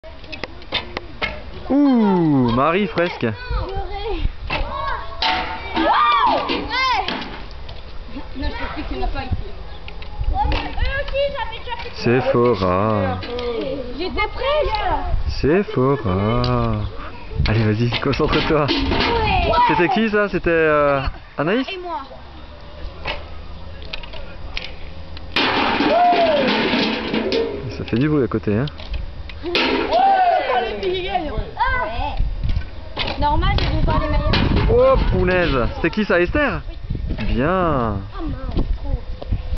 2007 Rouen - Tir à l'arc à Jumiège - Filles
Pendant la colo, les activités ne manquent pas.

Cet après-midi la, le groupe des filles est opposé aux garçons pour un concours de tir à l'arc à la base de Jumiège.